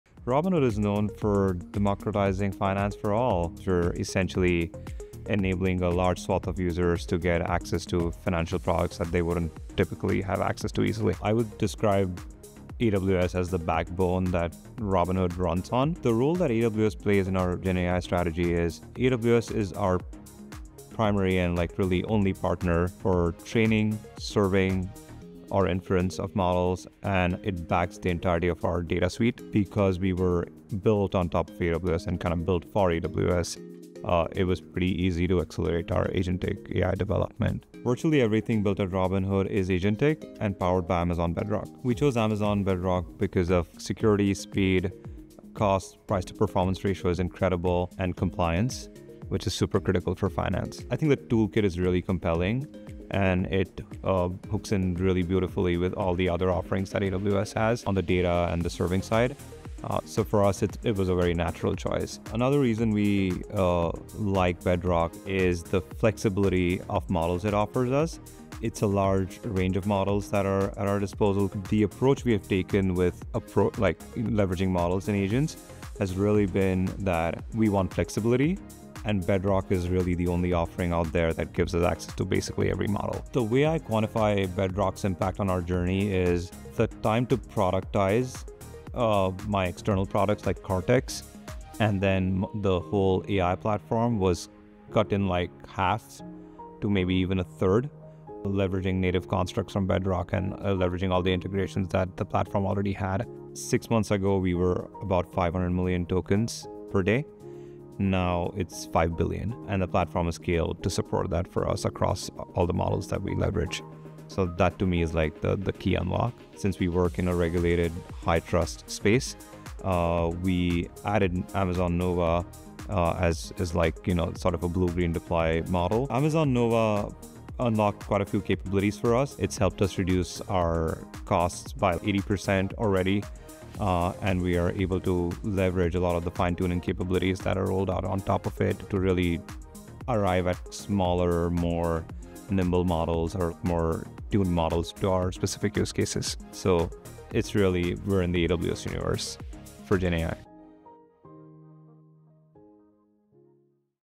Robinhood democratizes finance with Amazon Bedrock | Amazon Web Services
Robinhood has evolved from a fintech disruptor to an AI-first financial innovator, with virtually everything now built using agentic AI through Amazon Bedrock. In just six months, the company scaled from 500 million to 5 billion tokens daily while cutting AI costs by 80% and reducing development time by half. Dev Tagare, Robinhood's Head of AI, explains how Amazon Bedrock's diverse model selection, security, compliance, and price-to-performance advantages make it ideal for the regulated financial sector. Amazon Nova has been particularly transformative for Robinhood, offering multimodal capabilities and fine-tuning options that enable more specialized models. Robinhood's story showcases how fintech leaders are leveraging cutting-edge AI to democratize finance while maintaining critical security and compliance standards.

Why AWS?